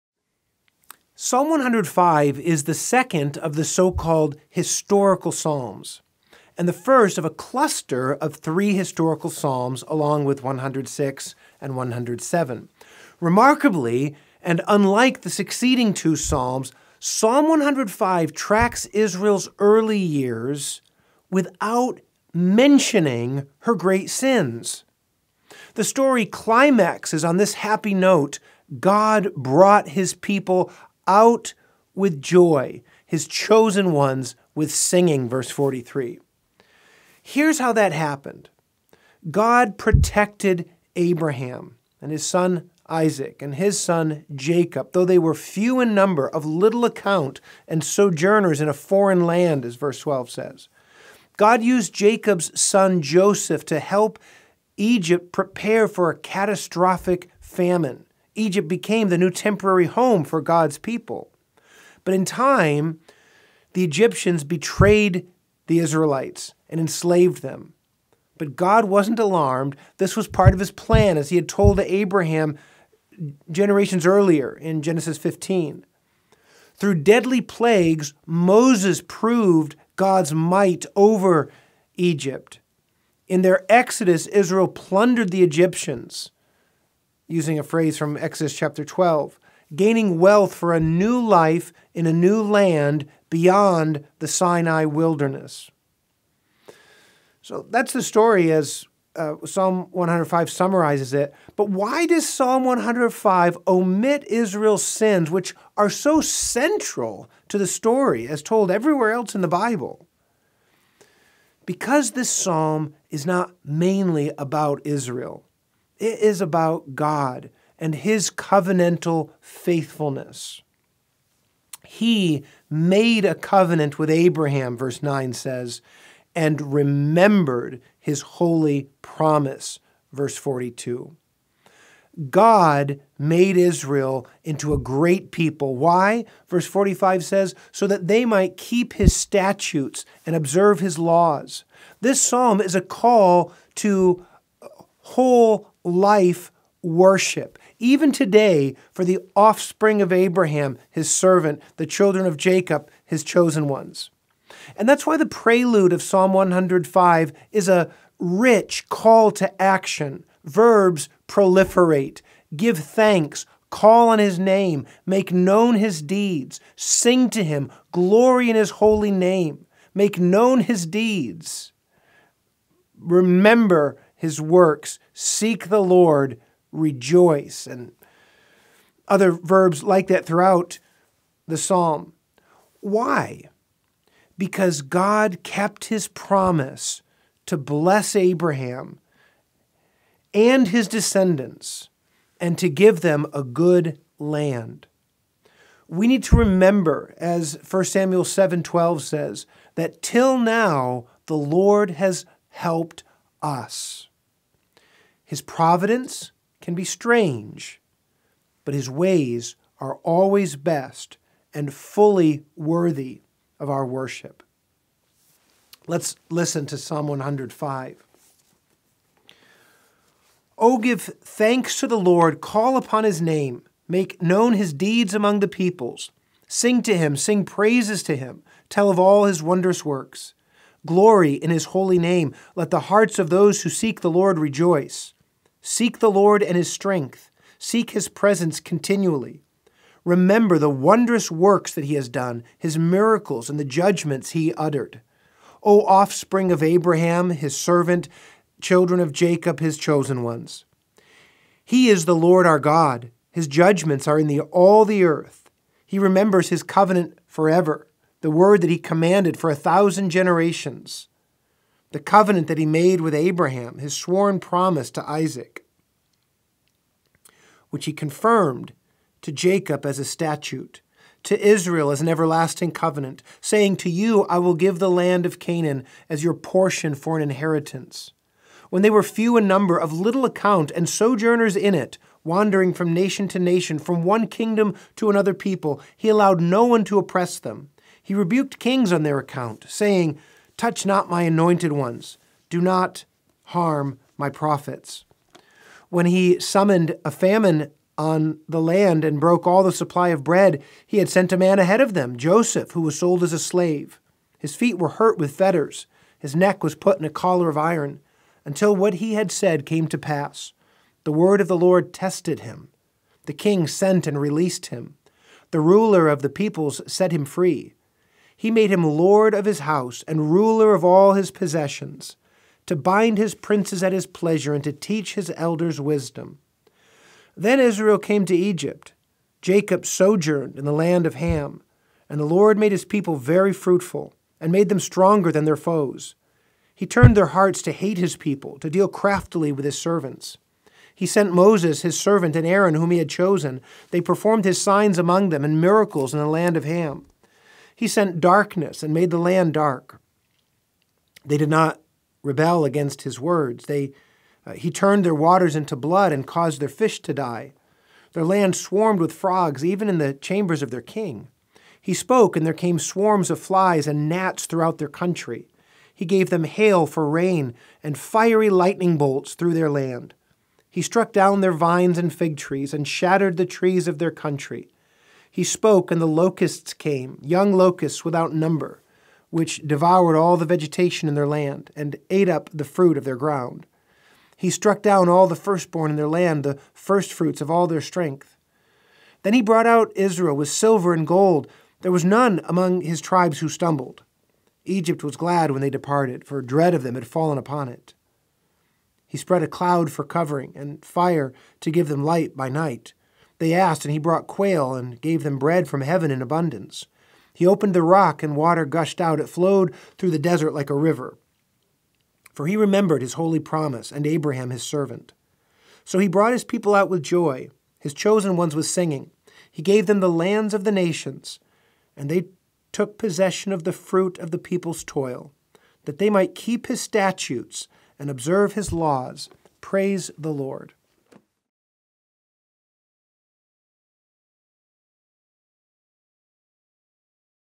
Psalm 105 Meditation - Remember God’s Faithfulness
Synopsis: Psalm 105 is the second of the so-called “historical psalms,” and the first of a cluster of three historical psalms along with 106 and 107 (see also 114, 135–136). Remarkably, and unlike the succeeding two psalms, Psalm 105 tracks Israel’s early years without mentioning her great sins. The story climaxes on this happy note: God “brought his people out with joy, his chosen ones with singing” (43).
Here’s how that happened. God protected Abraham, Isaac, and Jacob, though they “were few in number, of little account, and sojourners” in a foreign land (12). God used Jacob’s son Joseph to help Egypt prepare for a catastrophic famine; Egypt became the new temporary home for God’s people. But, in time the Egyptians betrayed the Israelites and en-slaved them. But God was not alarmed; this was part of his plan (Gen. 15:13–14). Through deadly plagues Moses proved God’s might over Egypt. In their exodus Israel plundered the Egyptians (Ex. 12:36), gaining wealth for a new life in a new land beyond the Sinai wilderness.
So why does Psalm 105 omit Israel’s sins, which are so central to the story? Because the psalm is not mainly about Israel; it is about God and his covenant faithfulness. He made a covenant with Abraham (9), and “remembered his holy promise,” (42).
God made Israel into a great people “that they might keep his statutes and observe his laws” (45). This psalm is a call to worship—even to-day—for the “offspring of Abraham, his servant, children of Jacob, his chosen ones! (6). The prelude is a call to action. Verbs proliferate: give thanks, call on his name, make known his deeds, sing to him, glory in his name, rejoice, seek the Lord, remember his works (1–6). Why? Be-cause God kept his promise to bless Abraham and his descendants and to give them a good land (7–11; Gen. 12:3, 7). Remember that, “Till now the Lord has helped us” (1 Sam. 7:12). His providence can be strange. But his ways are always best.